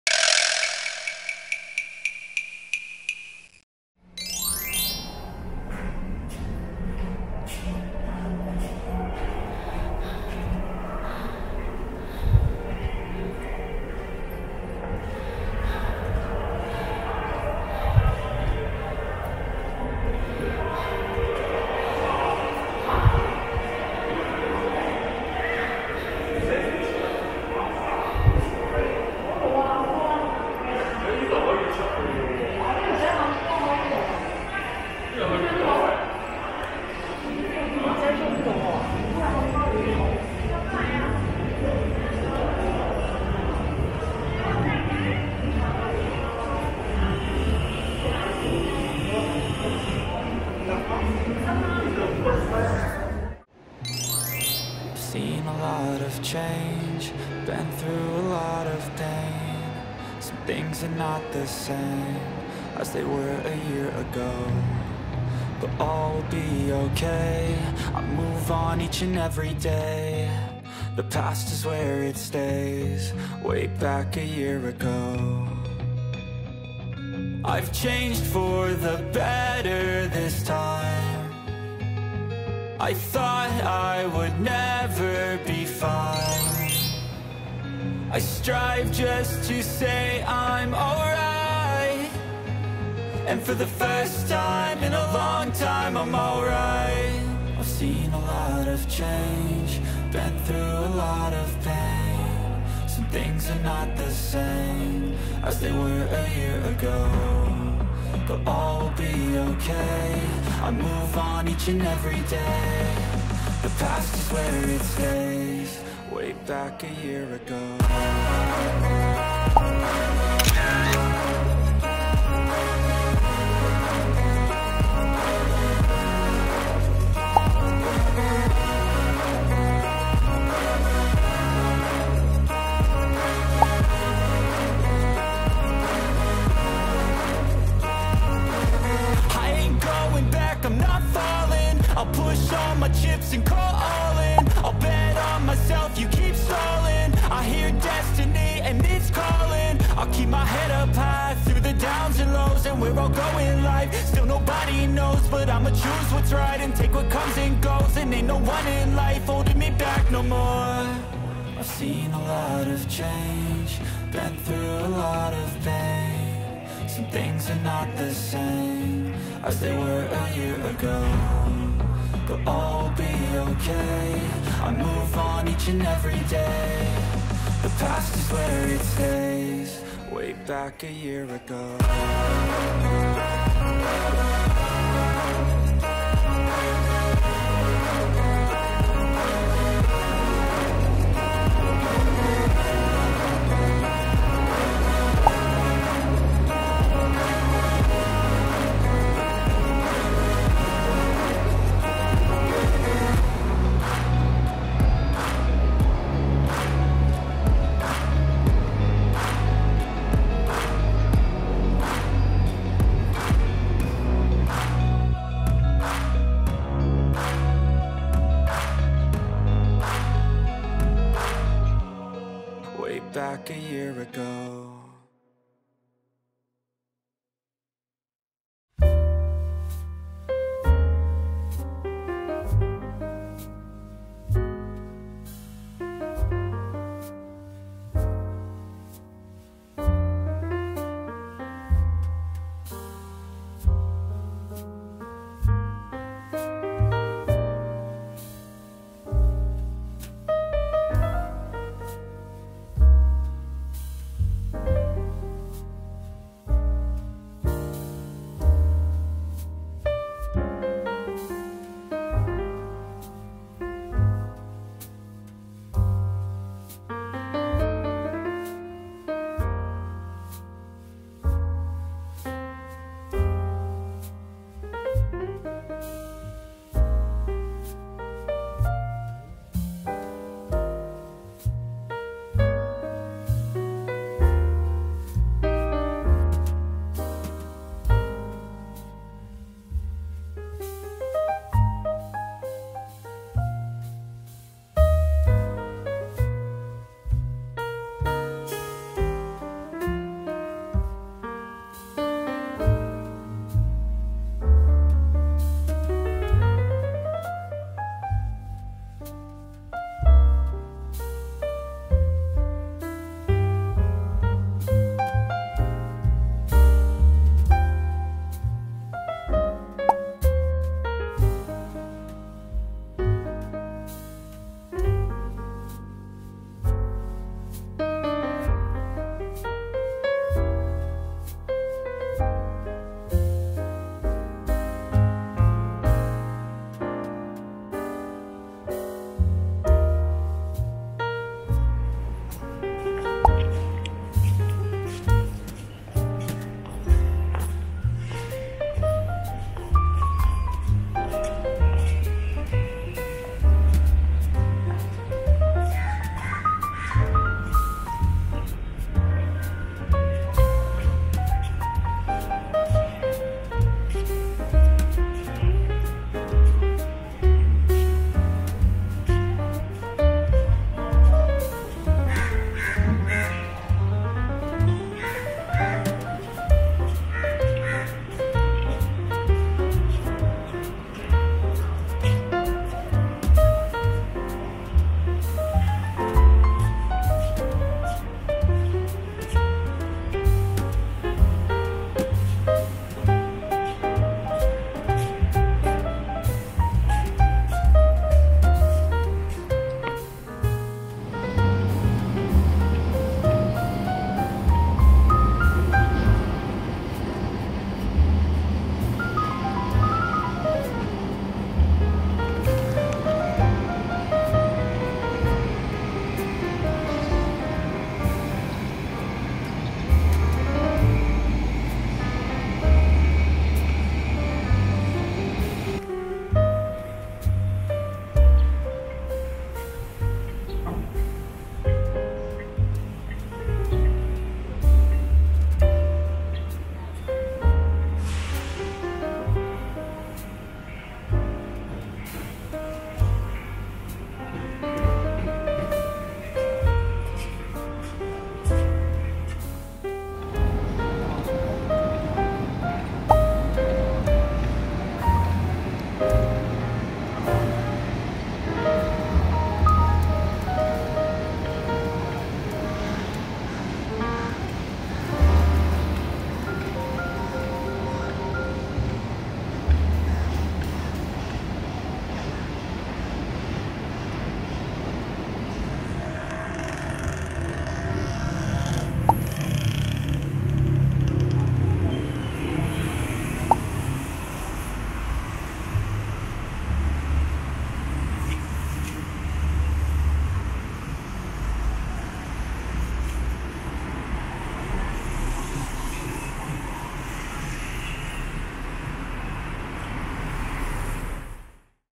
Modern Superior Japanese at Lalaport KL BBCC Shopping Mall Kuala Lumpur Preview - Nitori Nojima
You Know What Let's Look Mall Kuala Lumpur Preview Malaysia Experience With Latest Trends, Commercial Business No Need To Travel Far As It Bring Japanese Closer Here As Very Good Future For Both Countries